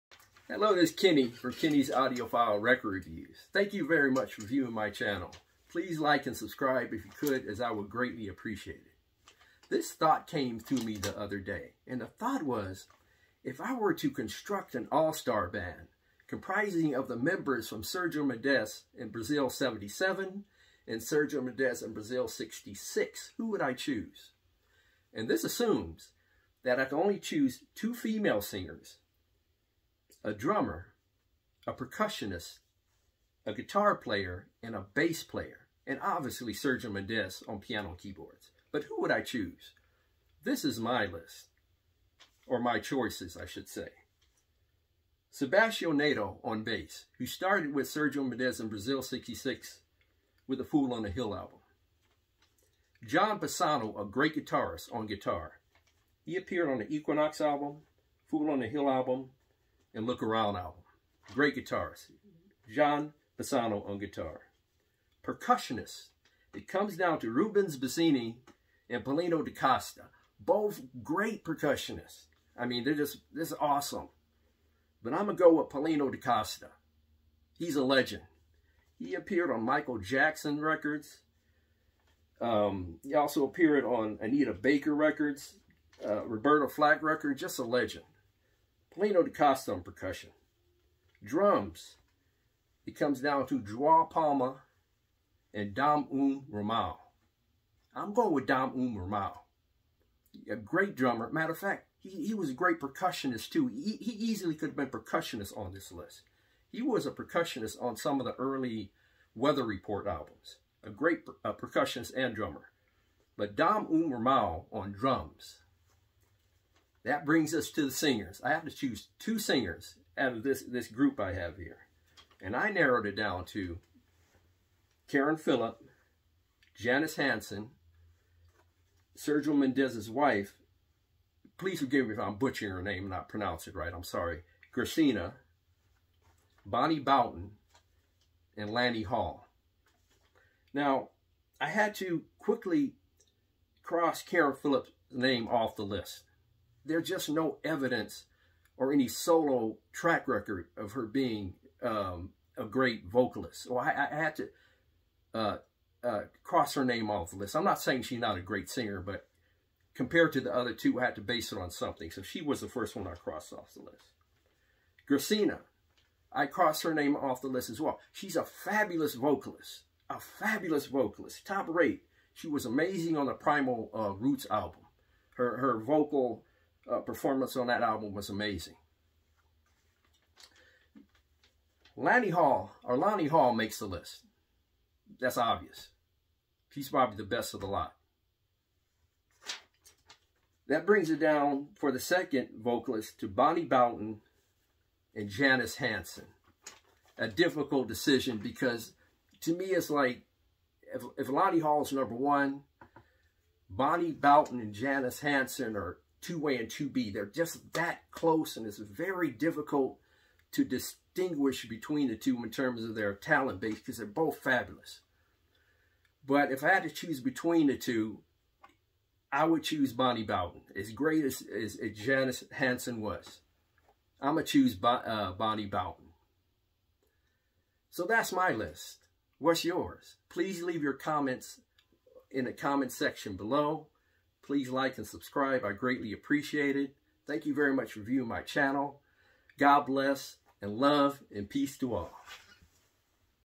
Picking a Sergio Mendes Brasil 66 & 77 ALLSTAR band
Picking a all-star band from the members of Sergio  mendes Brasil 66 and Brasil 77 bands